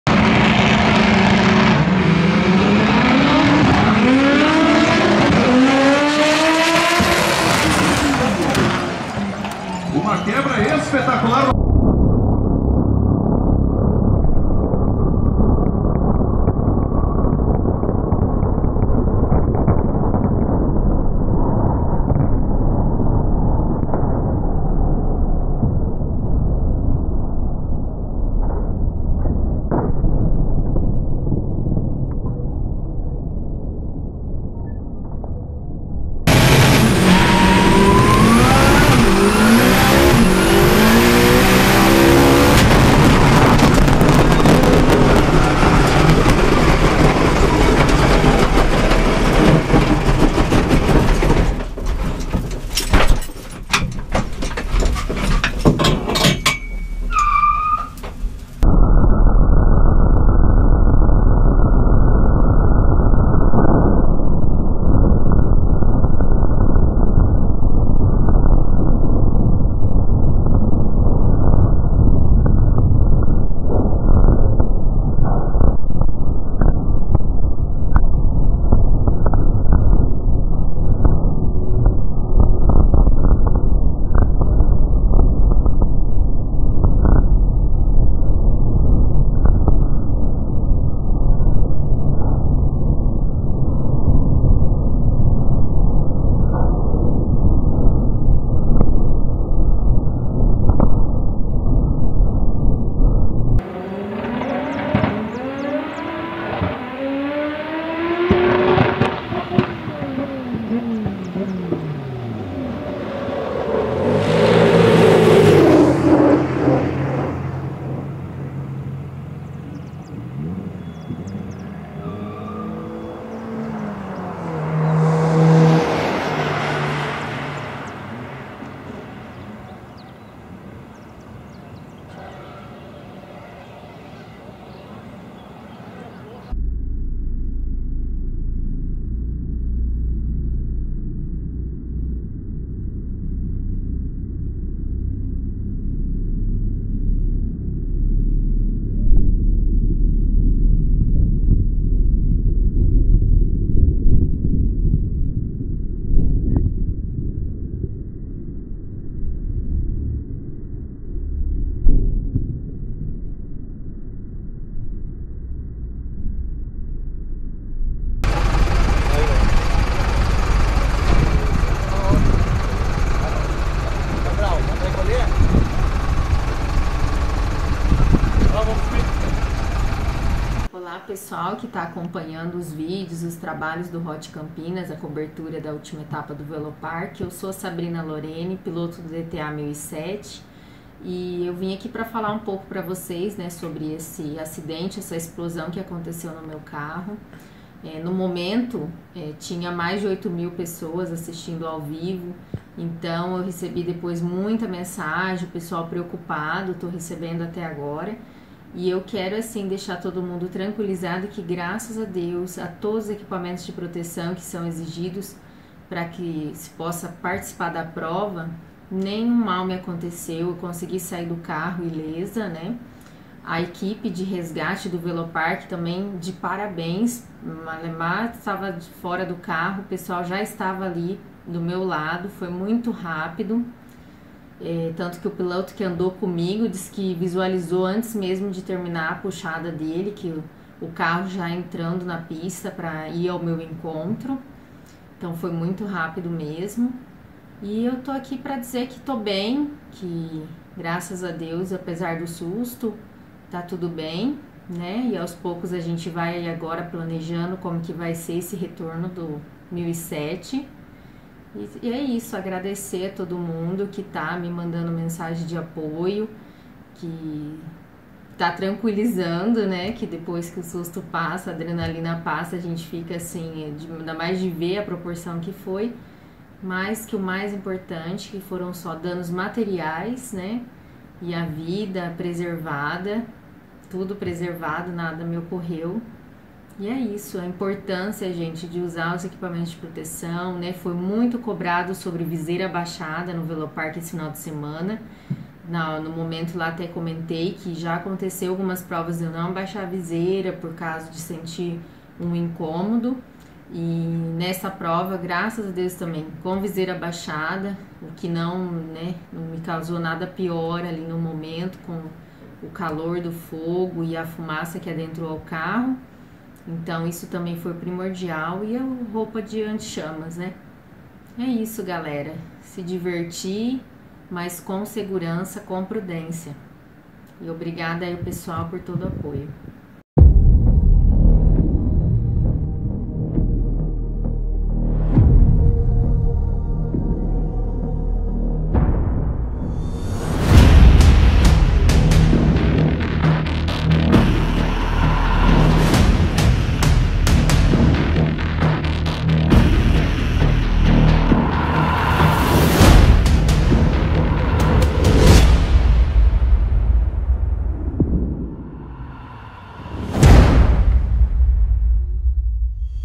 EXPLOSÃO - Espetacular do GOL AP 8V Turbo da Piloto Sabrina Lorene no Velopark
1ª 2ª 3ª BOOOOMMMM, Sabrina estava vindo com parciais excepcionais infelizmente o bloco não suportou a enorme potência no AP 8V e o resultado você confere no vídeo, vale enaltecer o tremendo trabalho da equipe de resgate do Velopark, e também o tamanho da pericia da Sabrina, mesmo o carro em chamas conseguiu controlar evitando um acidente ainda maior. Sabrina nos deixou um depoimento no final do vídeo. TODOS Torcendo para o breve retorno, comentem demonstrando o seu apoio!

VISITEM